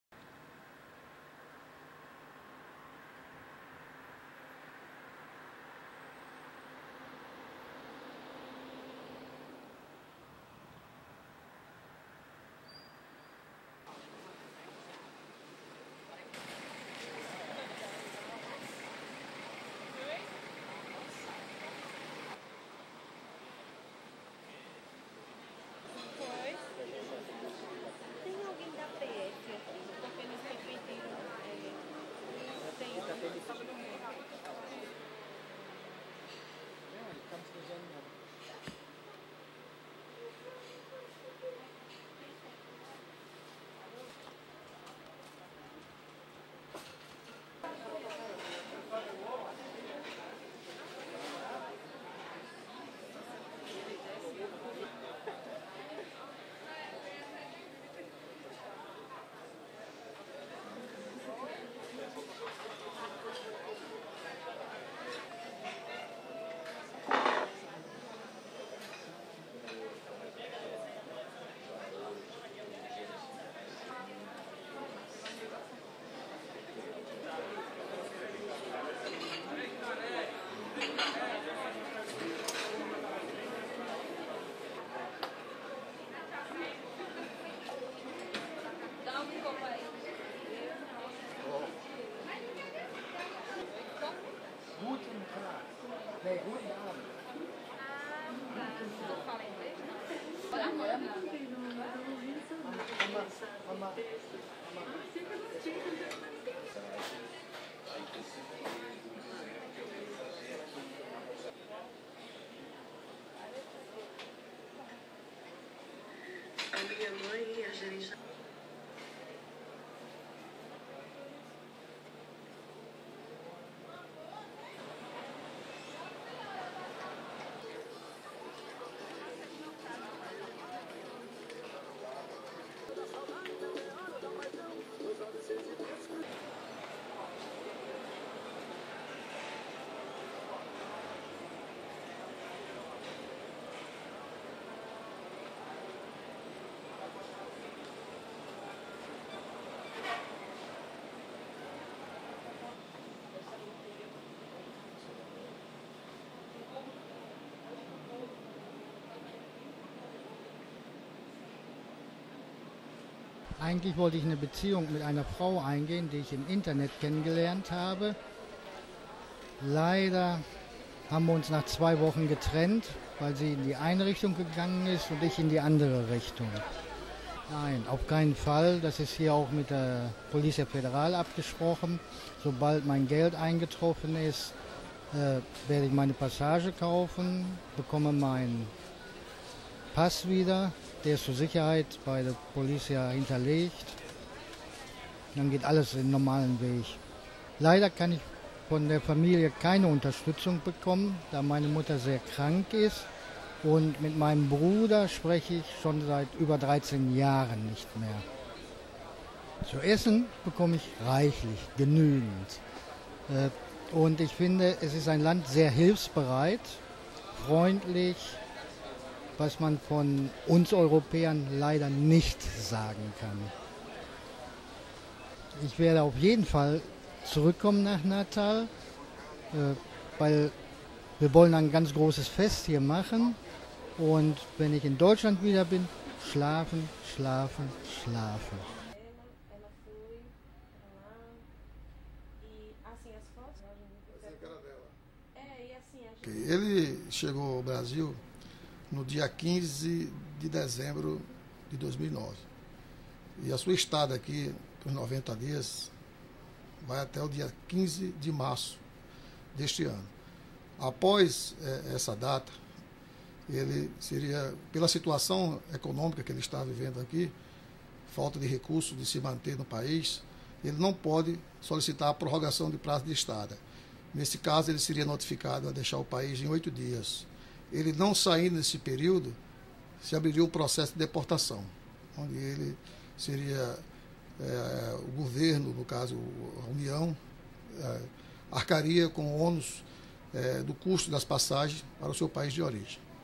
Morando no aeroporto.wmv
Um Turista Alemão morando no Aeroporto Internacional Augusto Severo em Natal/RN, Depois de uma decepção amorosa com uma moça que conheceu na internet ele foi rouba por bandidos em uma rua próxima ao hotel onde estava hospedado e ficou sem seus documentos e tambem seu bilhete de volta pra casa na Alemanha.